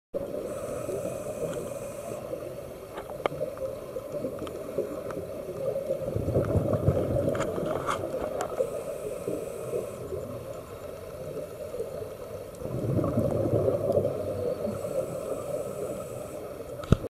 大きいクエに遭遇♪ブルーアース２１都立大のツアーにて♪「2018.2.20」
伊豆海洋公園にて出会った「クエ」。
実物は迫力がありました♪